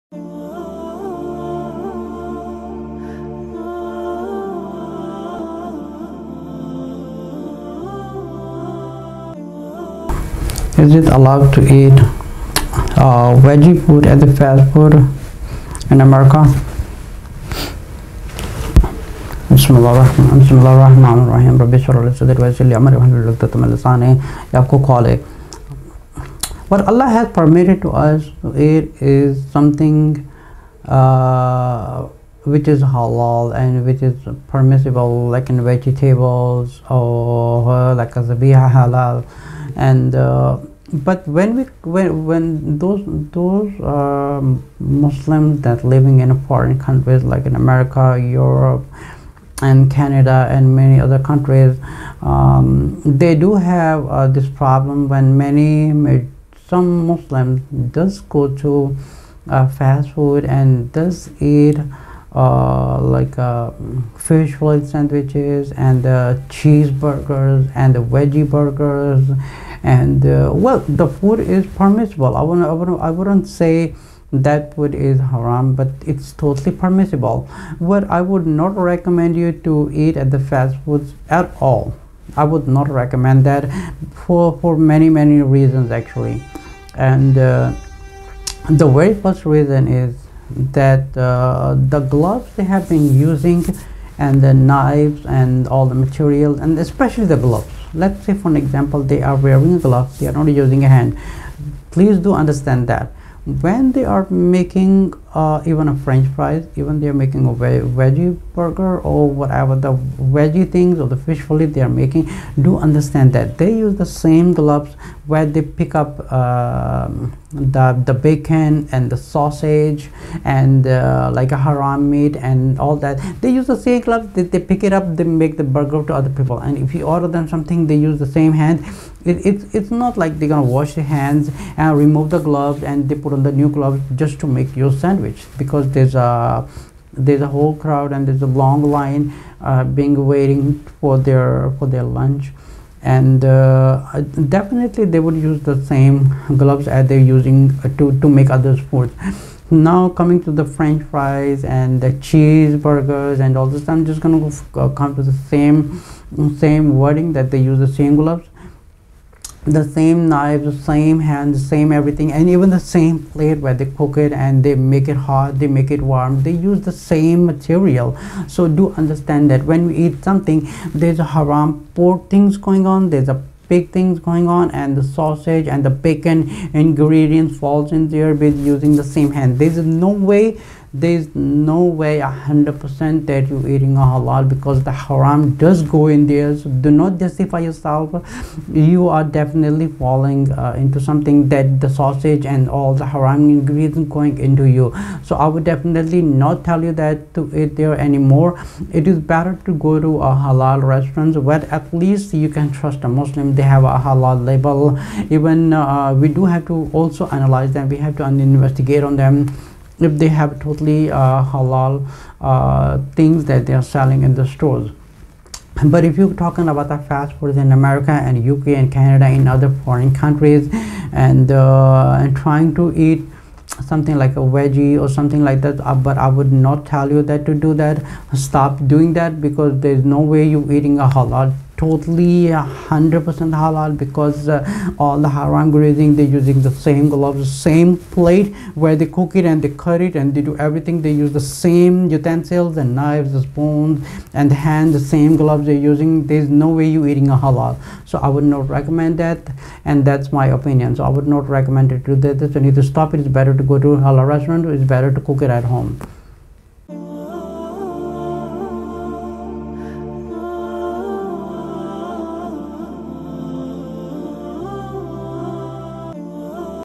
Can Muslim Eat McDonald Burger King Taco Bell Pizza Hut Subway Halal Haram? Mufti Ammaar Saeed
Mufti Ammaar Saeed
IRFNY